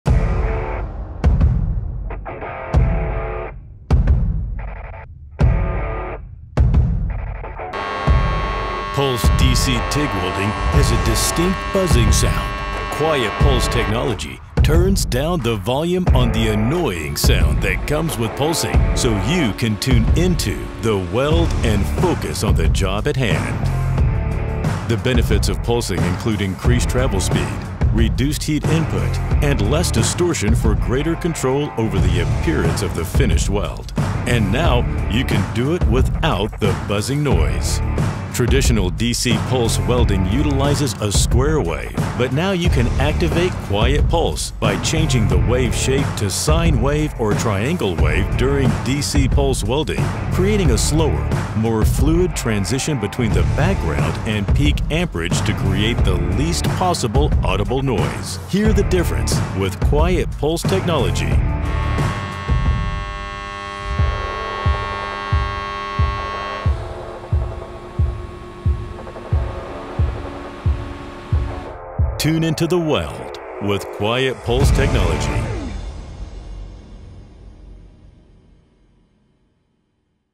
QuietPulse™ Technology for DC TIG Welding
Pulse DC TIG welding with QuietPulse™ Technology. Change your DC waveshape from square to sine or triangular to experience pulse TIG welding like never before.